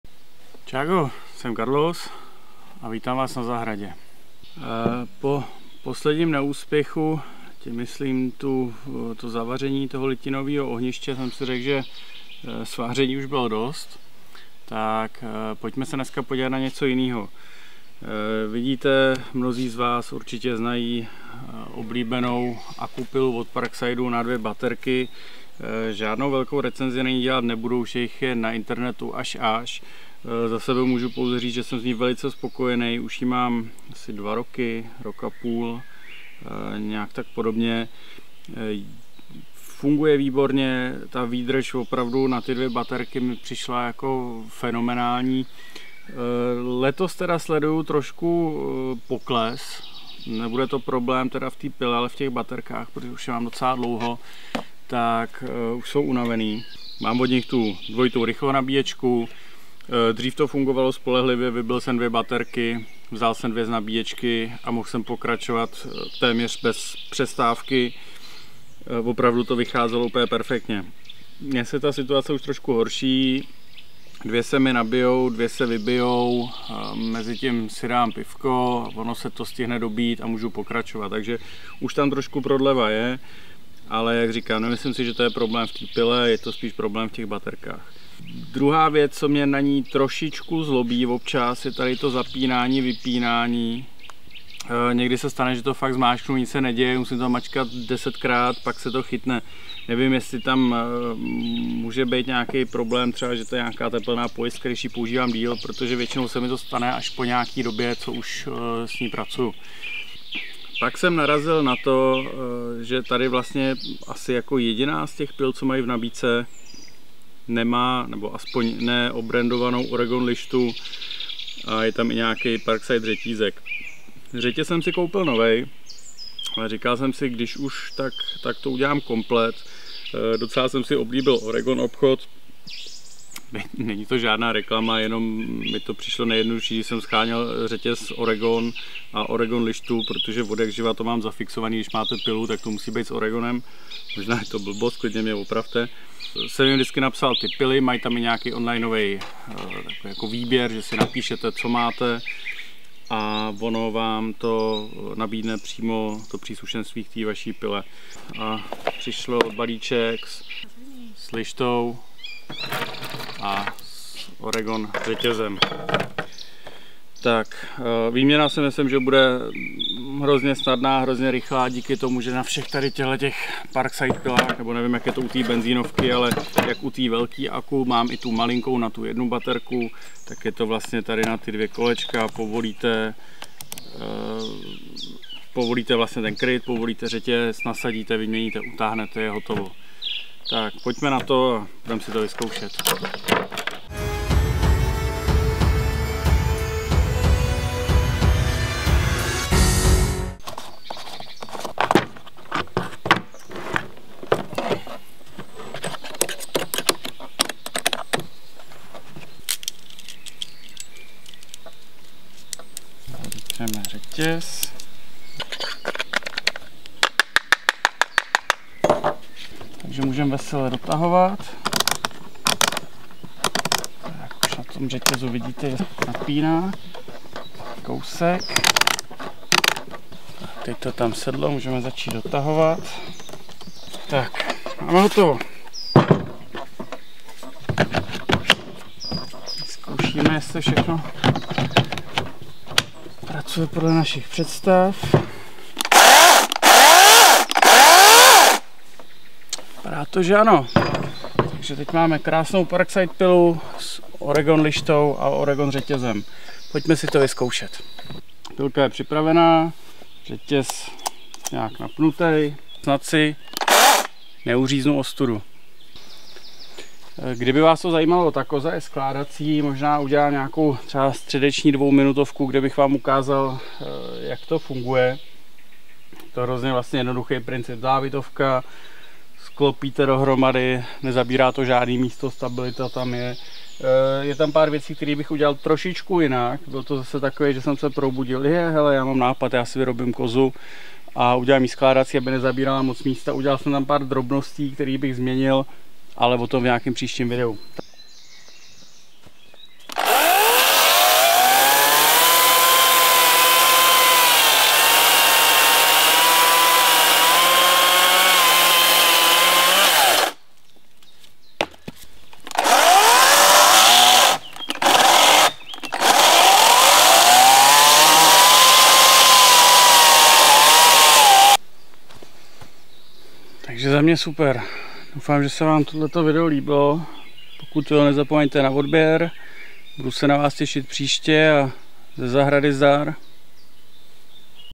Parkside akupila s oregon lištou? Proč ne?
Zdravím vás,
dnes jen kratší video, kde vám ukážu, že i na dvouakumulátorovou pilu parkside lze pořídit lištu od oregonu s jejich řetězem! Má to ale své ale, tak se mrkněte!

Čáu,
Karlos